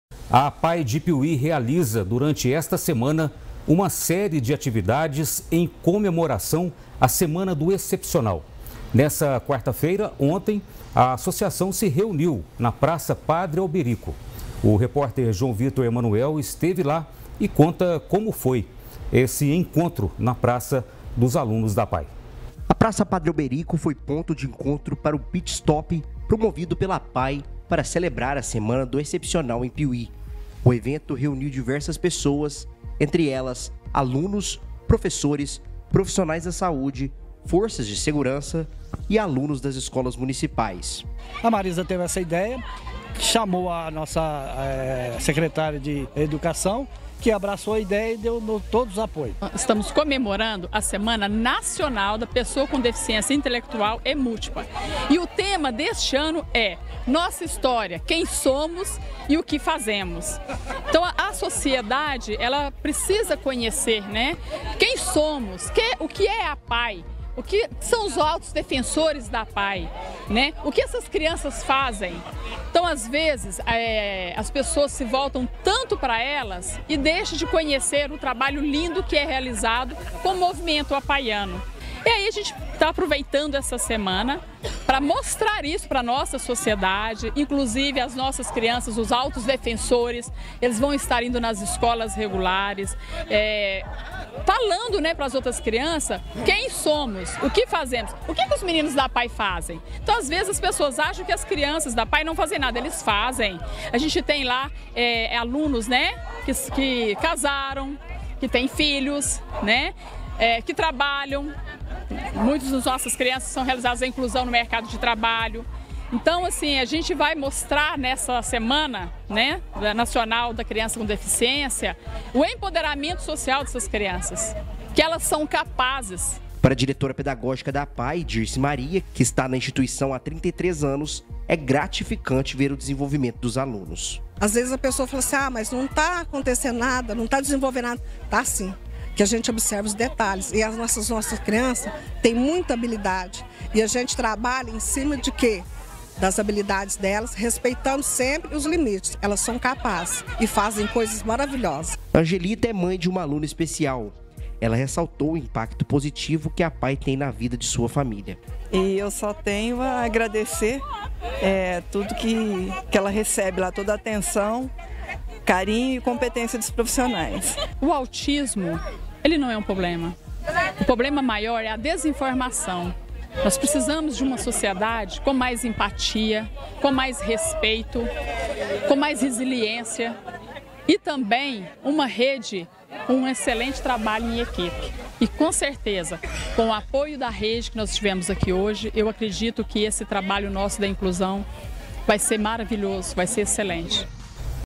APAE DE PIUMHI PROMOVE INCLUSÃO COM ATIVIDADES COMUNITÁRIAS NESTA SEMANA DO EXCEPCIONAL
| PIUMHI |

A APAE de Piumhi realiza, durante esta semana, uma série de atividades em comemoração à Semana do Excepcional. Nessa quarta-feira a associação se reuniu na praça Padre Alberico e o repórter João Victor Emanuel esteve lá e nos conta como foi, confira: